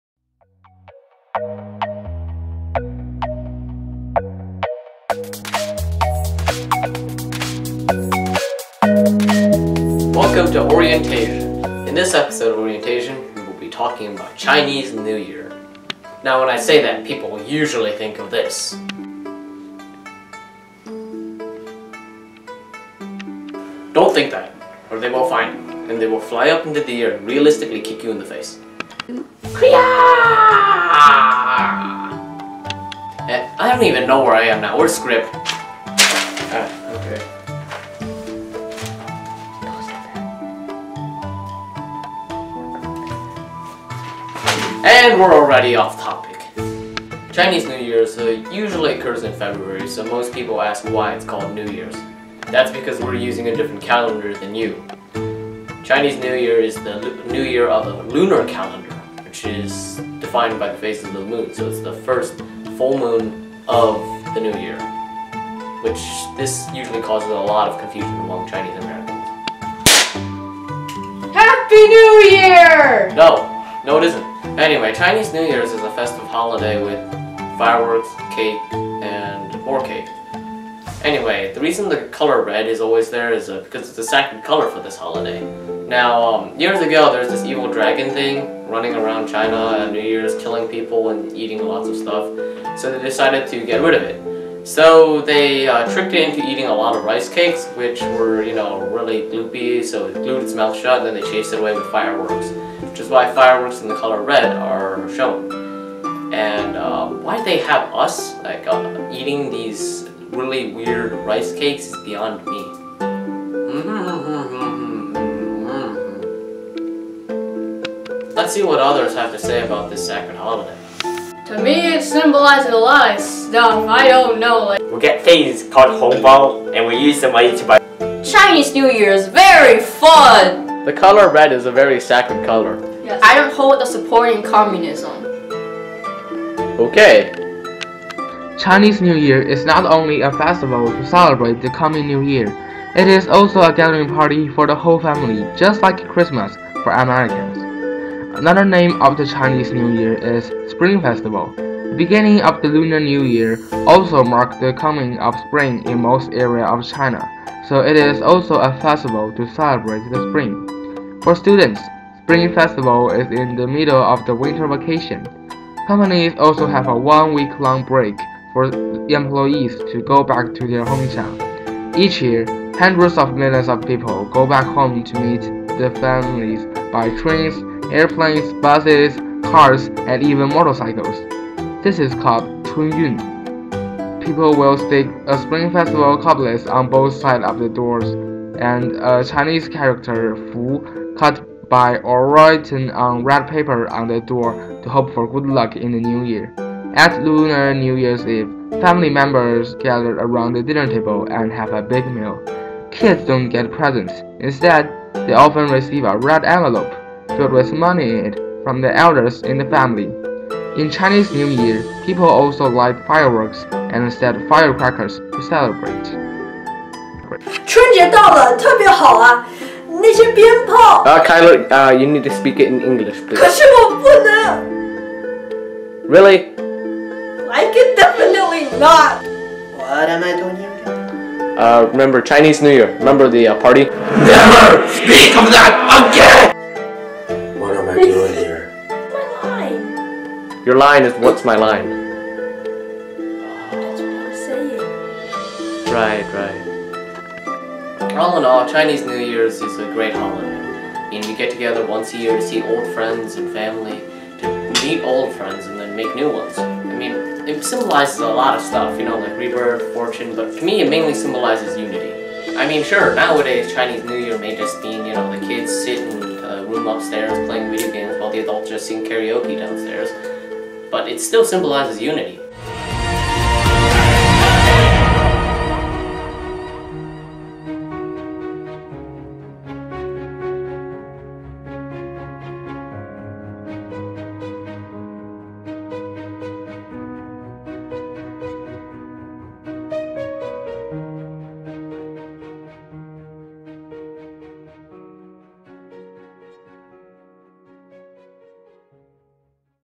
OrientASIAN: Chinese New Year
A short video about Chinese New Year. Thanks to everyone involved